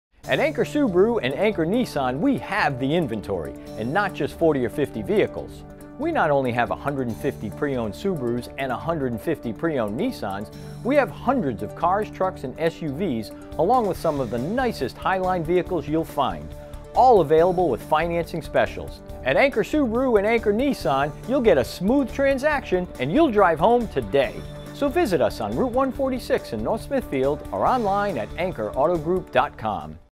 We Have the Inventory At Anchor Nissan
At Anchor Nissan we have the inventory and not just 40 or 50 vehicles! We we not only have 150 preowned Subarus and 150 preowned Nissans, we have 100s of cars, trucks, and SUVS, along with some of the nicest highline vehicles you'll find. All available with financing specials! At Anchor Nissan you'll get a smooth transaction and you'll drive home today!